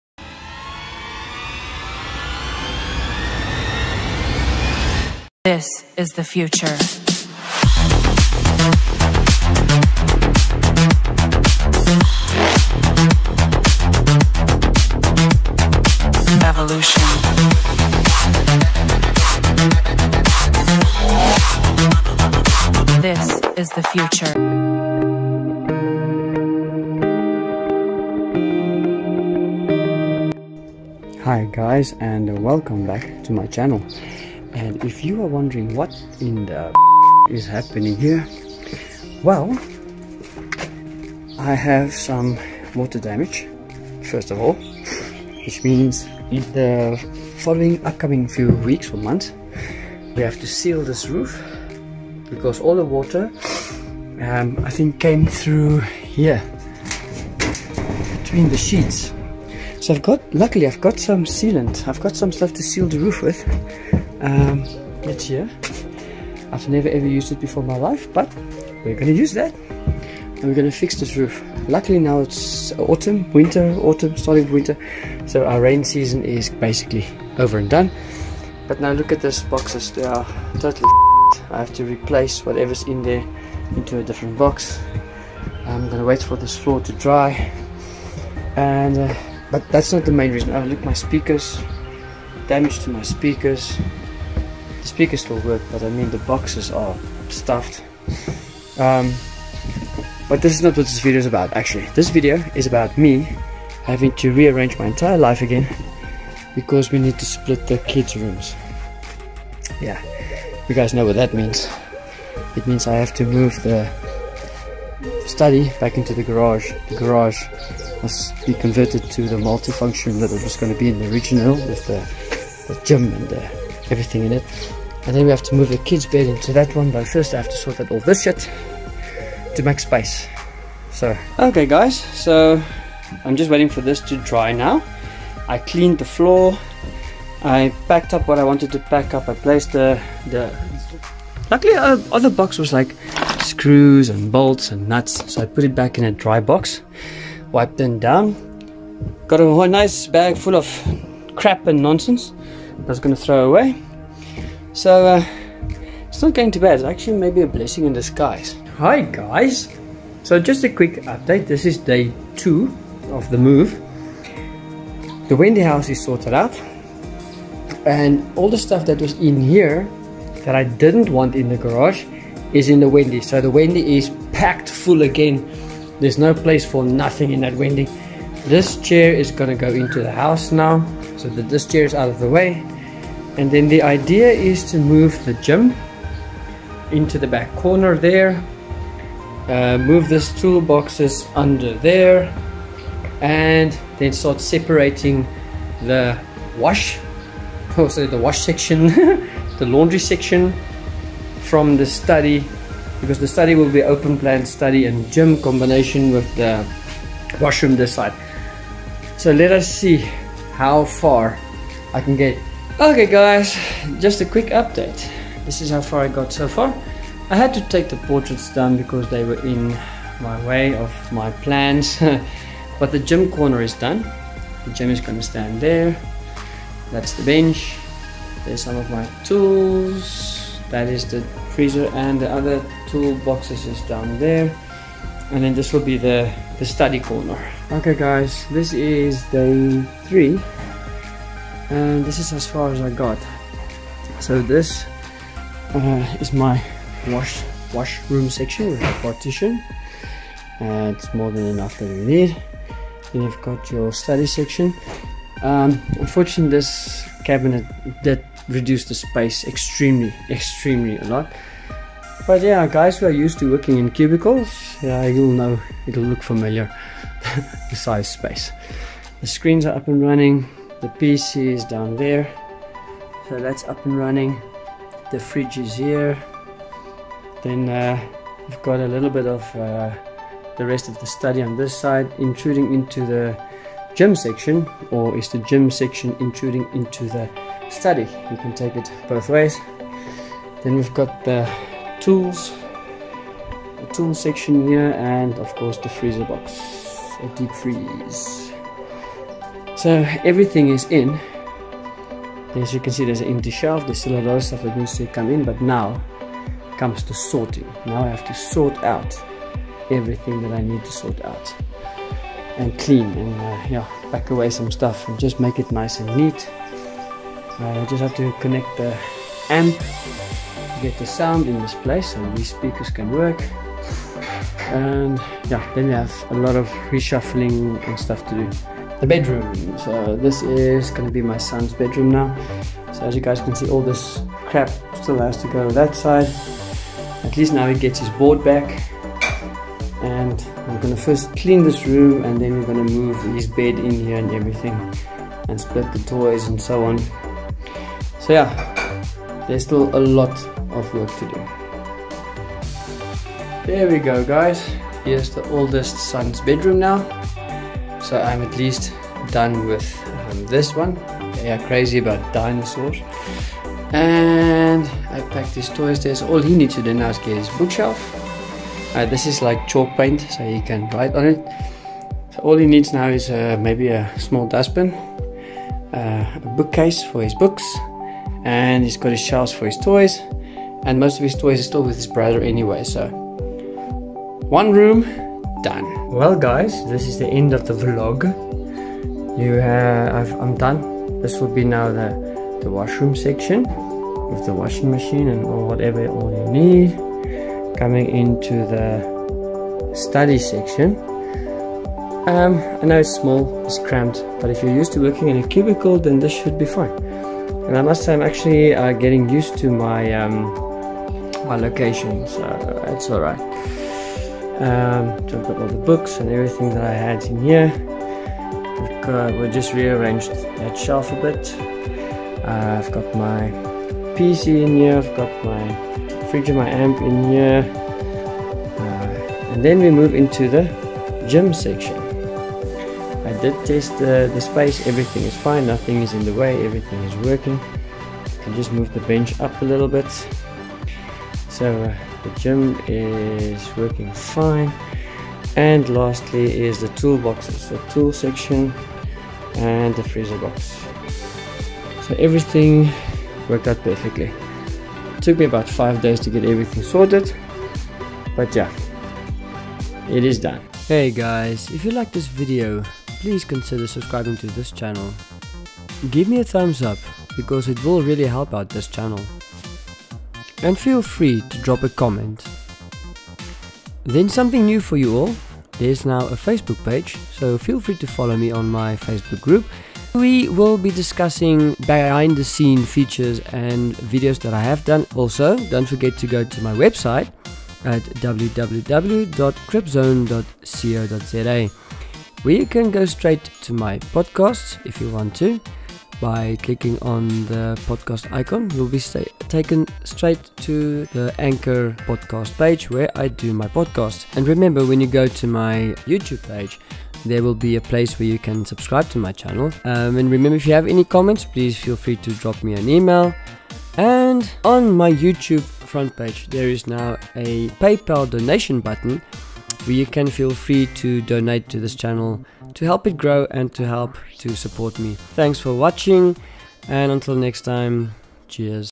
The move (vlog)
I had to give the kids each their own room so there was a major move that happened to get every thing sorted. This is just a Vlog. I had to get a video out.